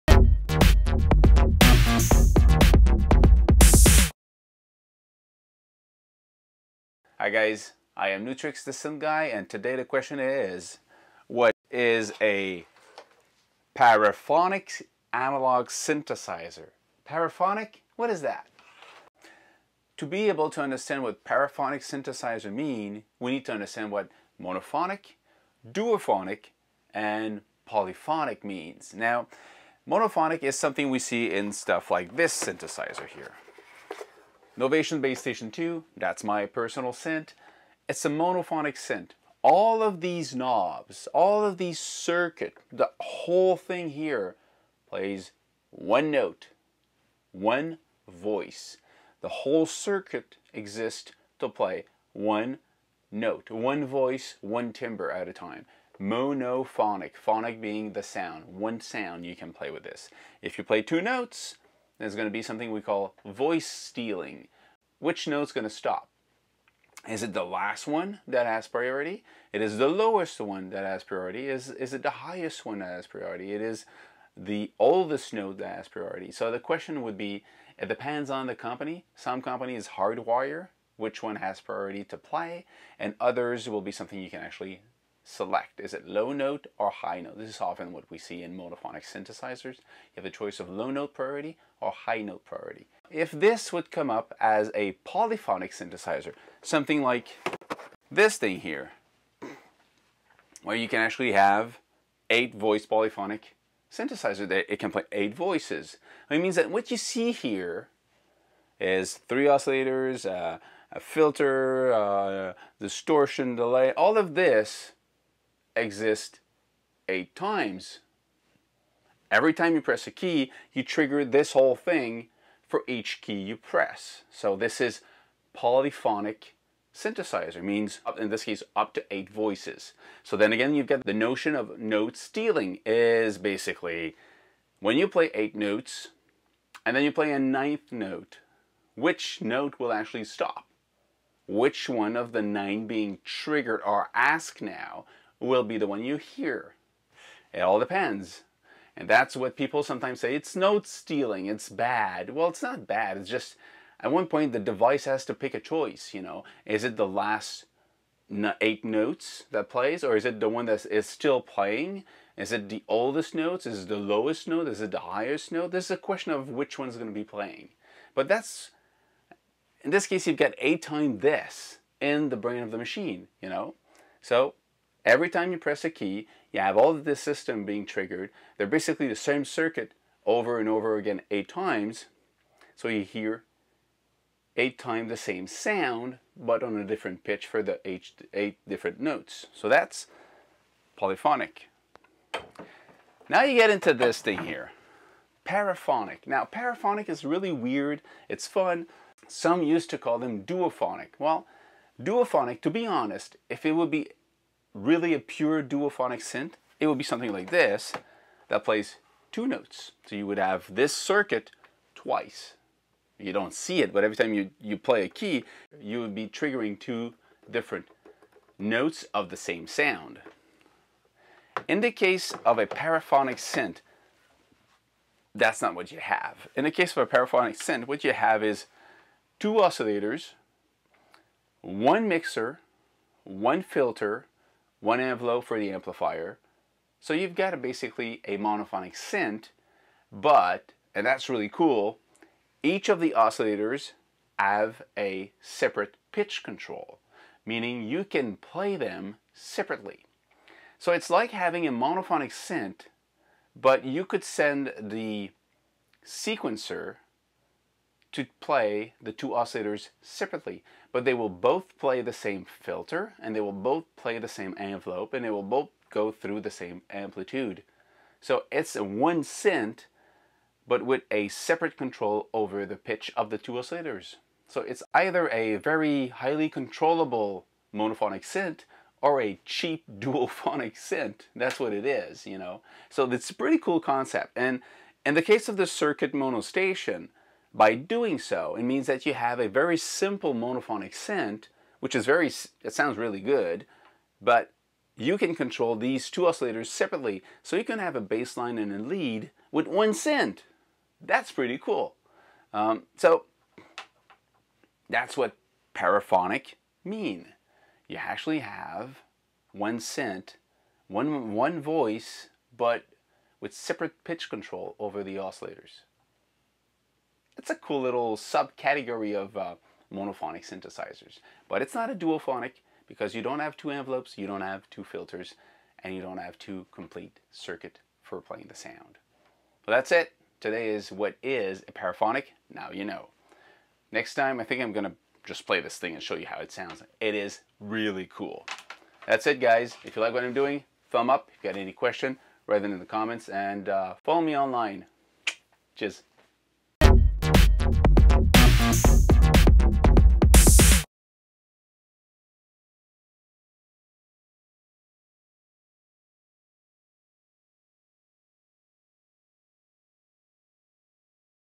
Circuit MONO Station : What is a PARAPHONIC Synth?
The Novation Circuit Mono Station is a really cool little piece of electronic music gear. It's simple and very efficient. It can be use as a Monophonic synth or a Paraphonic synth. What? Parapho... What? Yep, in this video I explain the concept behind paraphonic voicing in a synth.